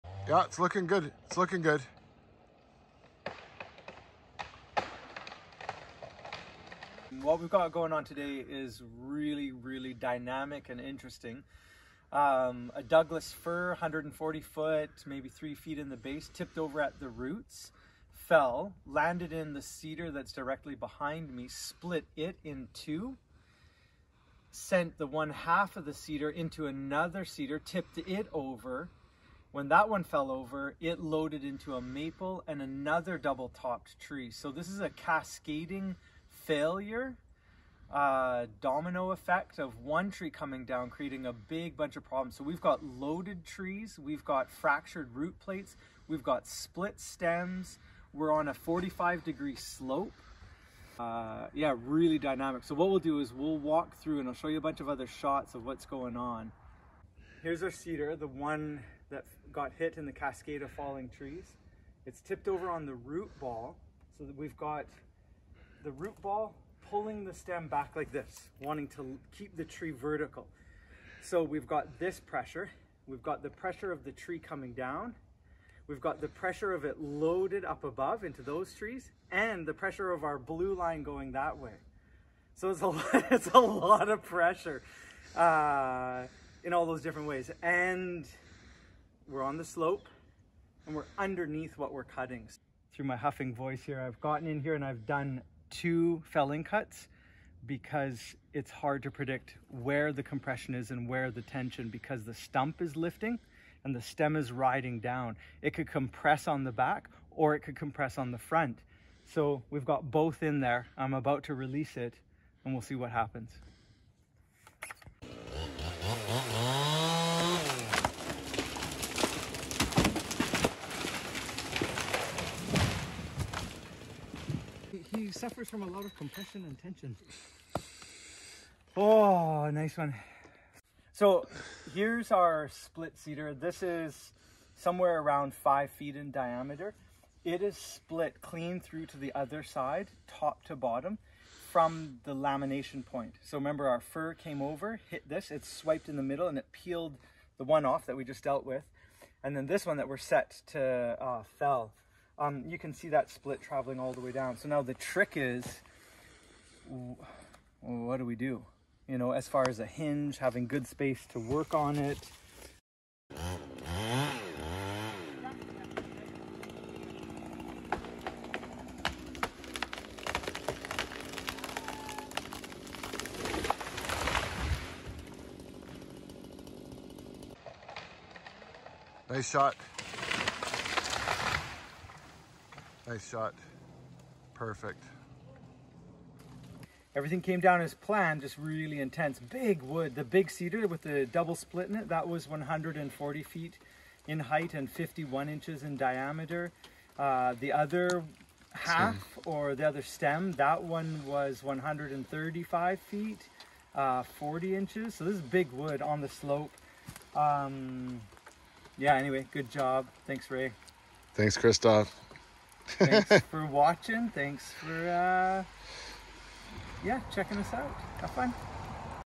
Domino Effect Tree Failure from Storm Damage
Extremely dangerous domino effect of tree failures that cascades into several compression and tension scenarios.  Watch the step by step process for a safe take down.